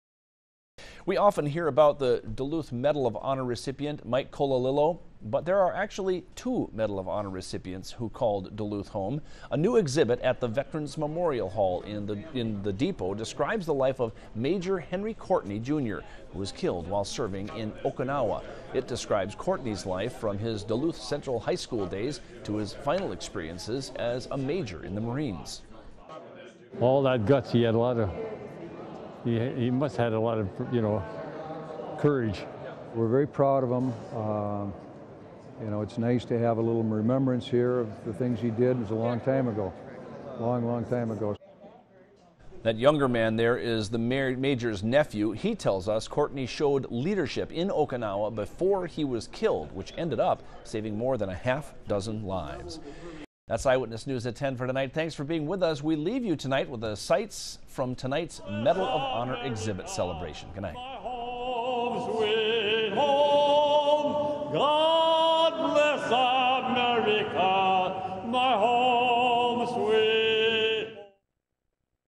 Medal of Honor Exhibit Celebration, at the Depot, Major Henry Courtney, Jr., in Duluth Minnesota
We often hear about Duluth Medal of Honor recipient Mike Colalillo. But there are two Medal of Honor recipients who called Duluth home.

A new exhibit at the Veteran's Memorial Hall in the Depot describes the life of Major Henry Courtney, Jr., who was killed while serving in Japan. It describes Courtney's life from his Duluth Central High School days to his final experiences as a Major in the Marines.

Nick Zobenica, a veteran from Chisholm, said "all those guts he had, he must have had a lot of courage."

Courtney's nephew told Eyewitness News "we're very proud of him. It's nice to have a little remembrance here of the things he did. It was a long time ago, it was a long, long time ago."

Courtney's nephew said he showed leadership in Okinawa before being killed, which ended up saving between 6 and 8 lives.